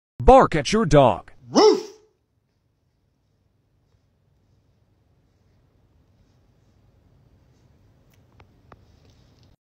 Keiko the dog reacts to owner barking 😂
Adrian from Arkansas decides to bark at her dog Keiko to see what its reaction will be. Keiko looks confused and shocked.